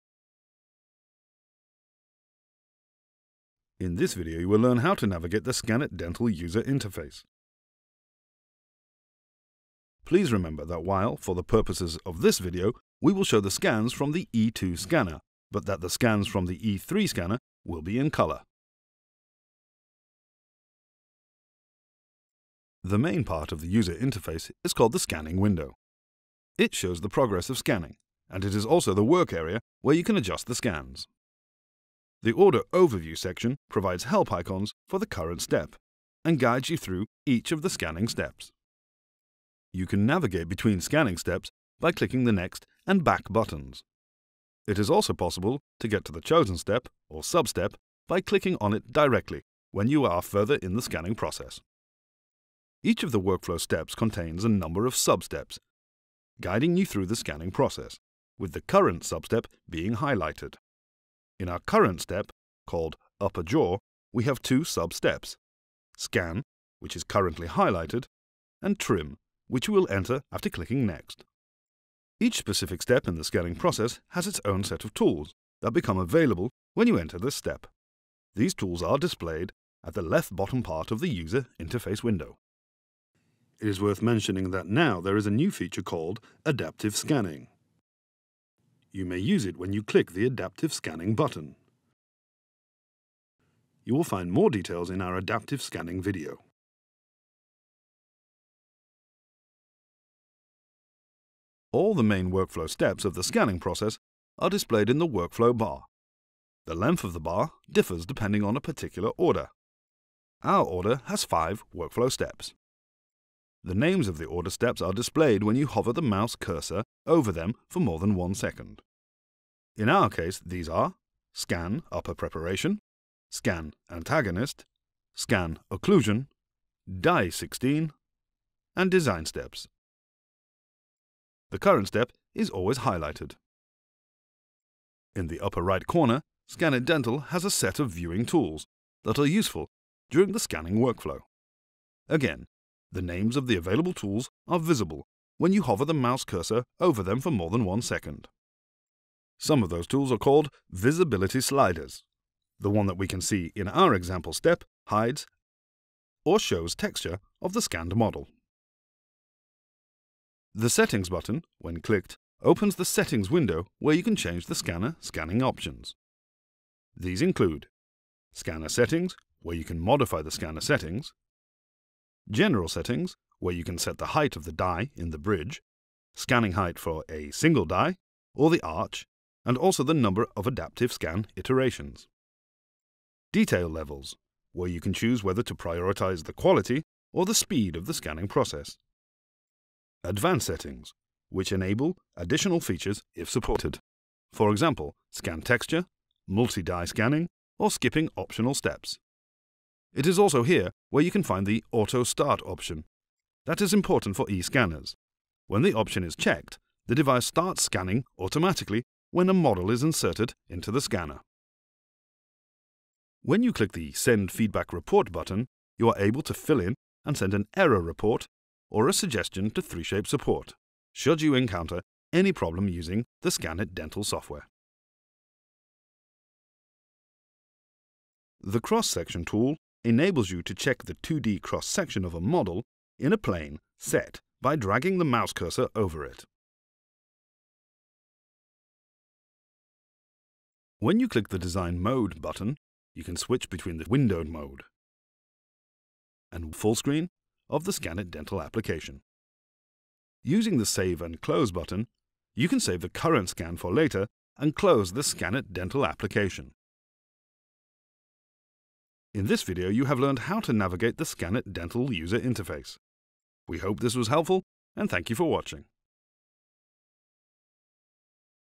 4. 3Shape E scanner - ScanIt Dental UI
The fourth video shows how to navigate the ScanIt Dental user interface, explaining all the components of the user interface and tools available during scanning. This includes the new adaptive scanning for plaster models option. The video also explains the scanning settings available for the user.